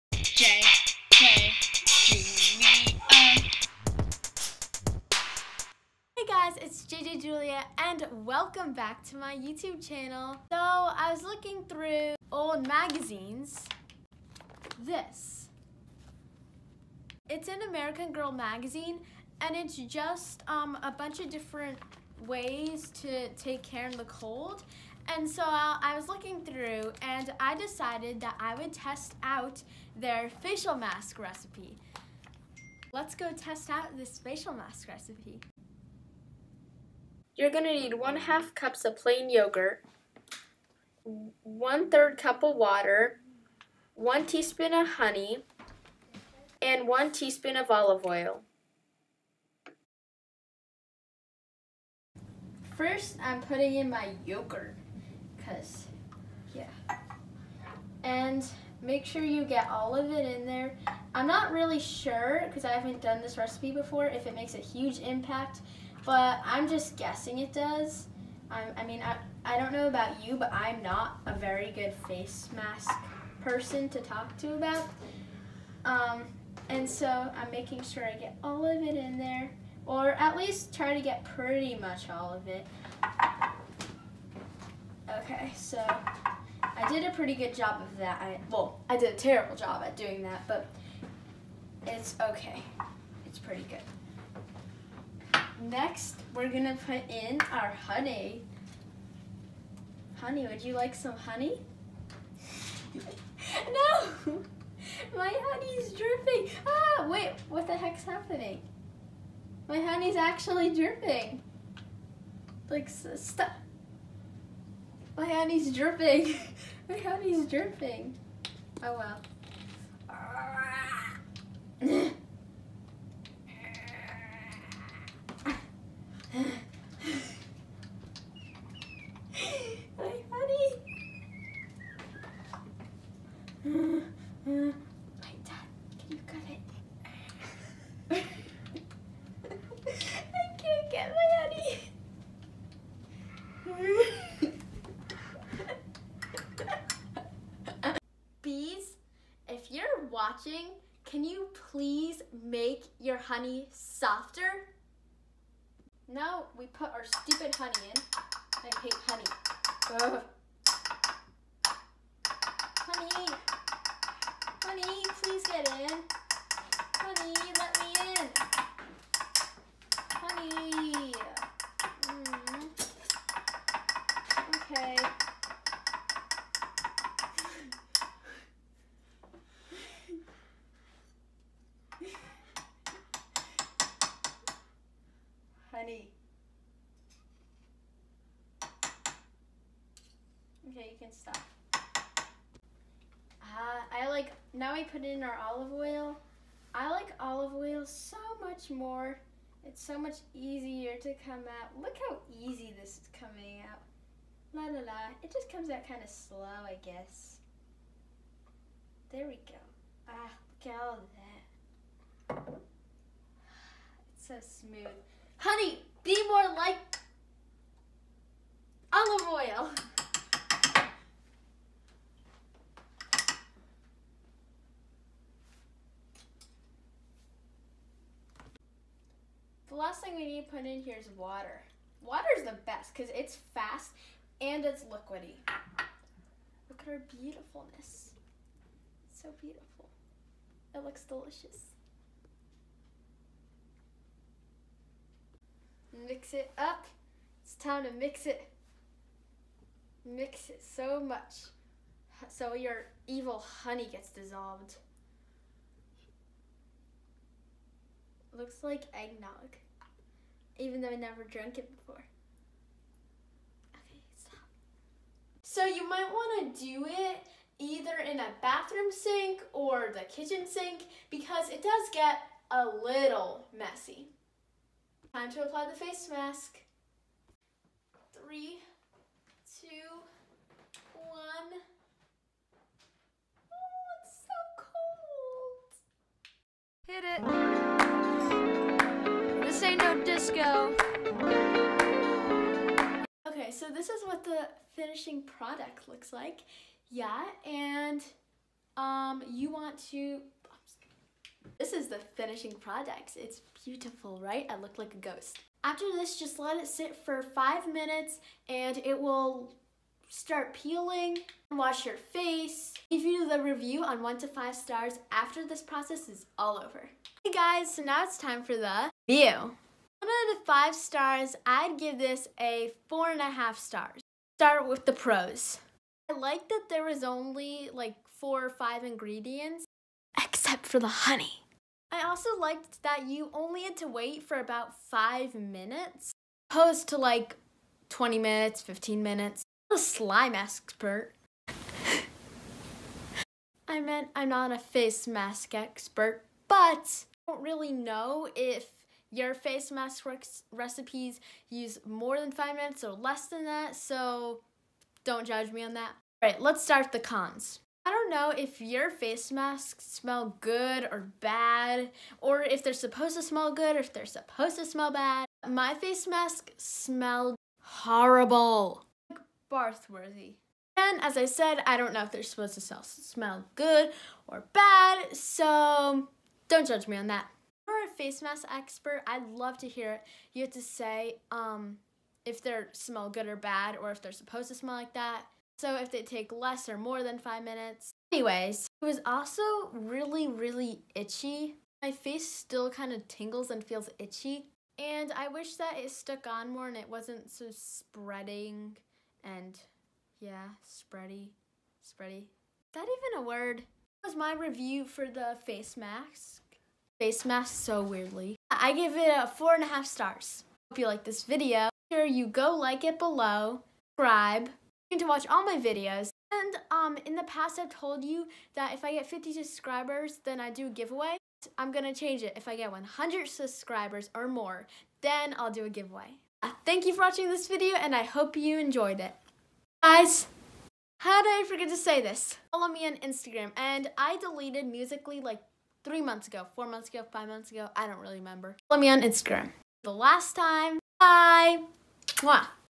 DIY face mask and review!
In this video I try out a face mask recipe from American girl magazine. In this video I forgot to say that the skin mask recipe can make about 5 face masks! perfect for parties with your girls! I hope you like this video more than I like honey! Bwahaha!! If I get 100 subscribers then I will do a giveaway!